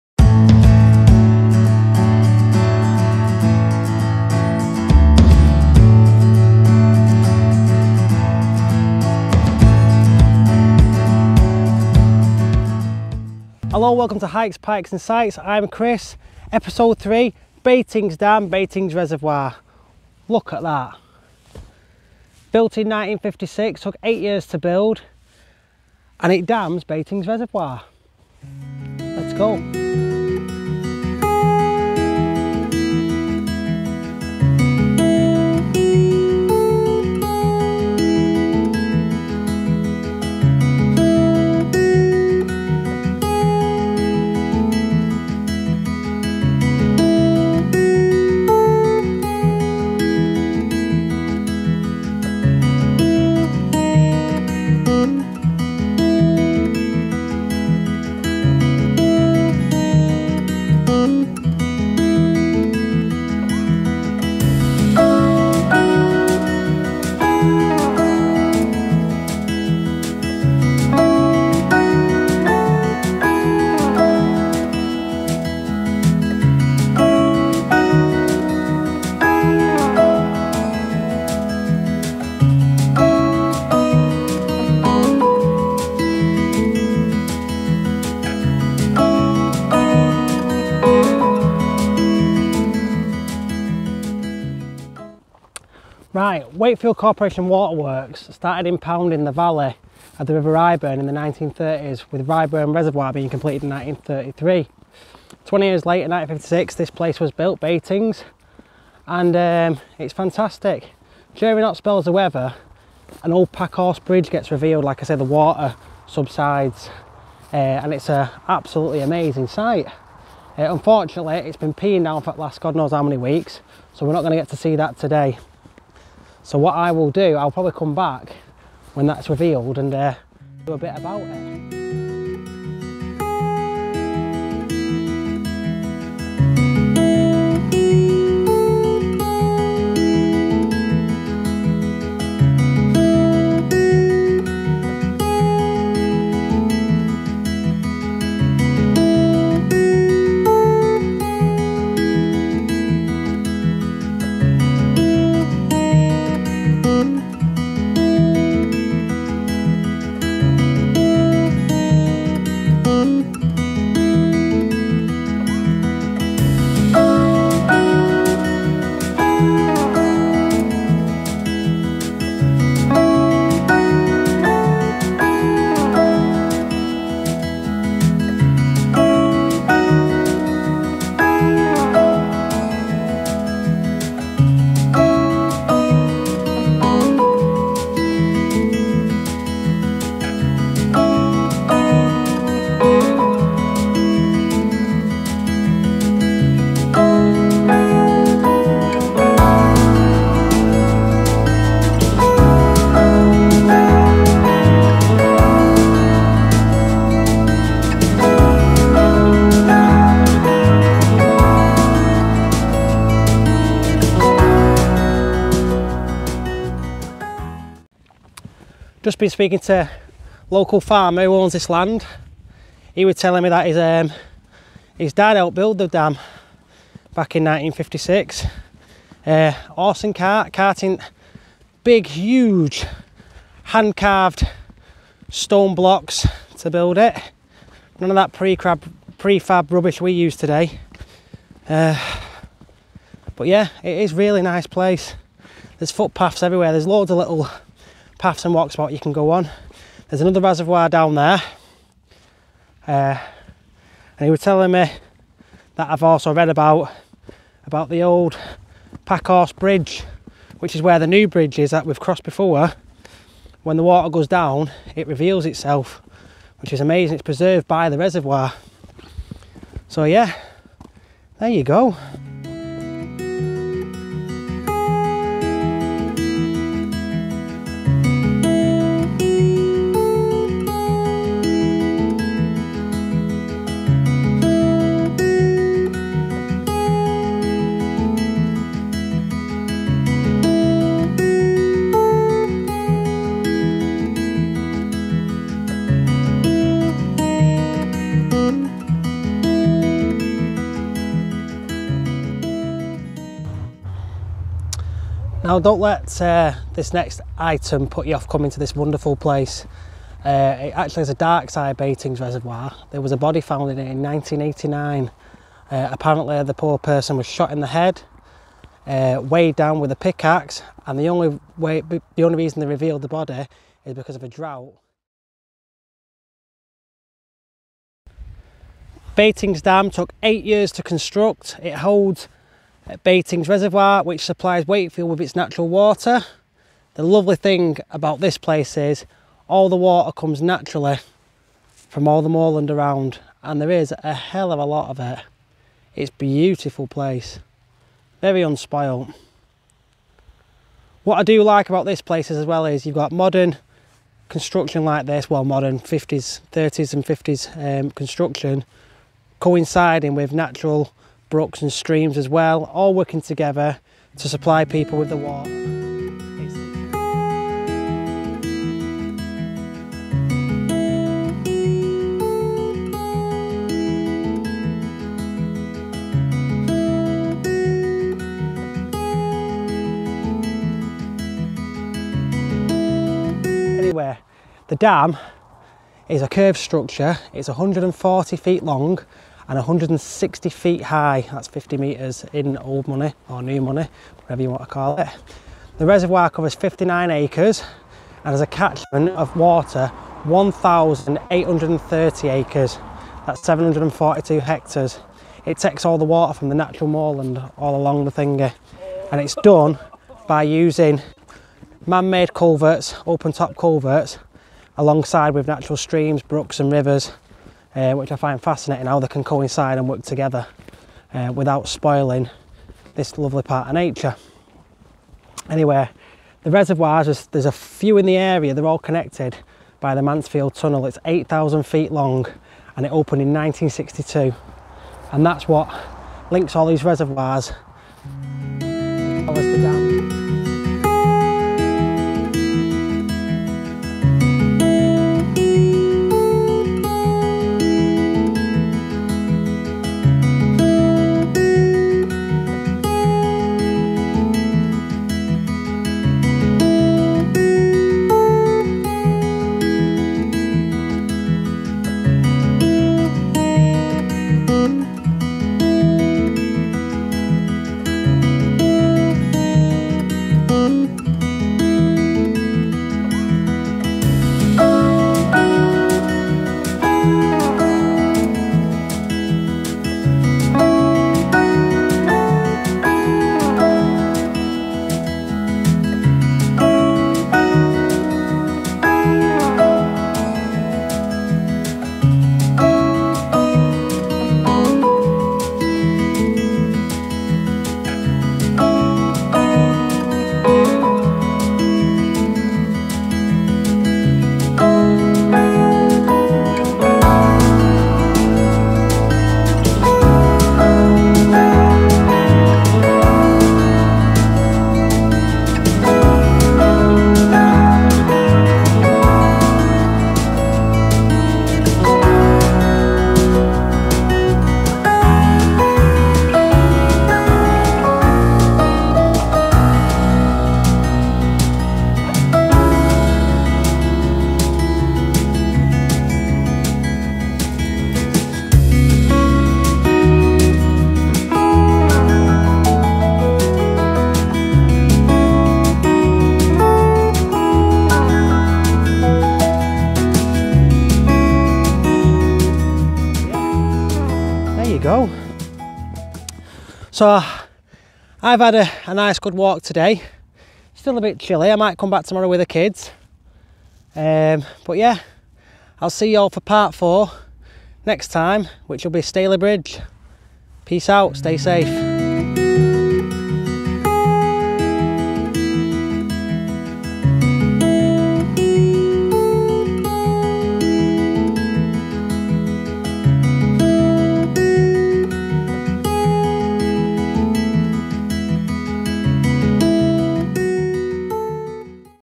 Beauty, Clever construction and Murder at Baitings Dam and Reservoir Vlog
Hikes, Pikes and Sights Episode 4 at Baitings Dam and Reservoir.
Really Enjoyed todays Vlogging, The amazing scenery and very Clever Classic British Construction at its best.

Wakefield Corporation Waterworks started impounding the valley of the River Ryburn in the 1930s, with Ryburn Reservoir being completed in 1933. Construction on Baitings took place 20 years later with completion in 1956. The reservoir takes the name of the hamlet that was flooded under the new dam. The hamlet of Baitings, whose name derives from the Old Norse of Beit (pasture) and English (meadow), was on an old road linking Yorkshire and Lancashire. During spells of very hot weather and drought conditions, the old packhorse bridge is revealed.

The dam head is a curved structure that is 1,540 feet (470 m) long and over 160 feet (50 m) high. The reservoir covers 59 acres (24 hectares) and has a catchment of 1,830 acres (742 hectares), and when it is full, it holds over 113,000,000 cubic feet (3,190,000 m3) of water. The dam took eight years to complete at a cost of £1.4 million, and is located at 840 feet (256 m) above sea level.  A tunnel connects reservoirs in valleys to the north with Baitings to allow for the transfer of water. Manshead Tunnel is 8,000 feet (2,400 m) long and was opened in 1962.

In 1989, the body of a man was found at the bottom of the reservoir during a period of dry weather, when the water was 40 feet (12 m) shallower than normal. He had been murdered and his body was weighted down with a pick axe. The crime was featured on Crimewatch and remains unsolved.

Equipment used
Lavalier wireles Mic
Samsung USB-C to Jack adapter
Bottle of water and a bar of chocolate ha ha ha